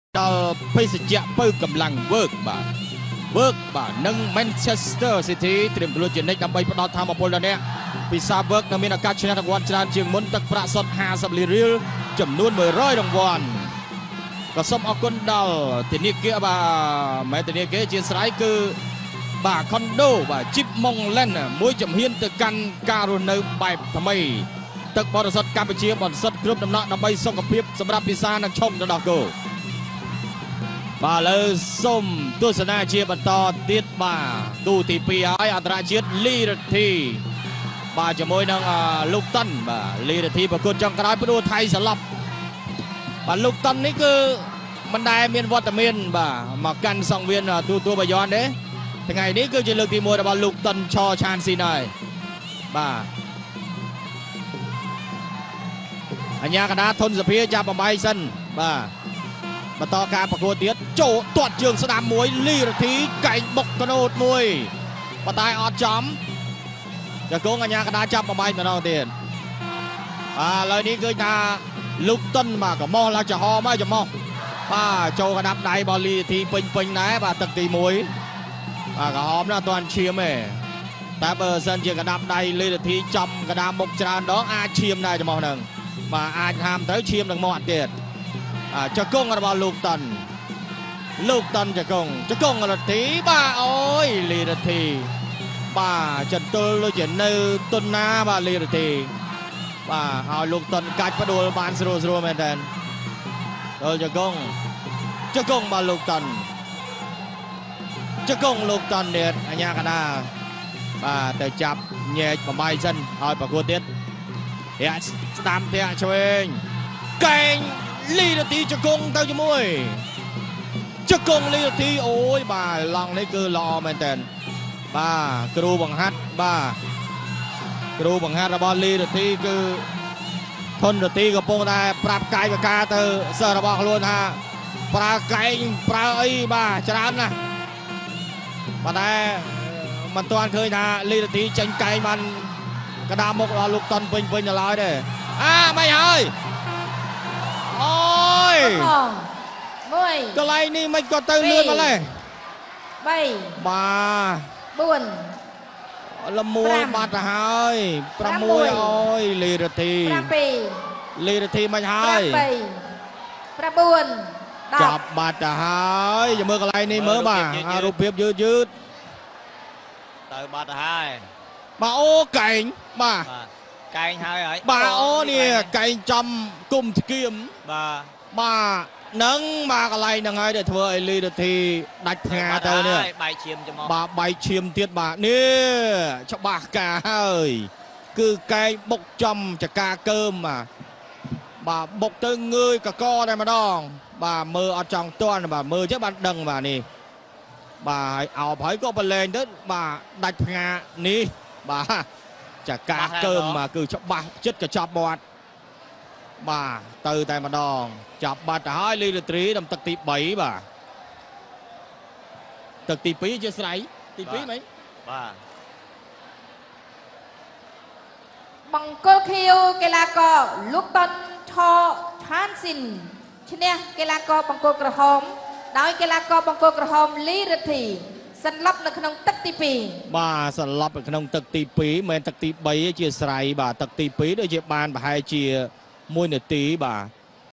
Ly Rithy vs LouktonThai, Bayon Khmer Boxing 15 Feb 2020, Kun Khmer vs Muay Thai
ប្រភពពី៖​Kun Khmer Top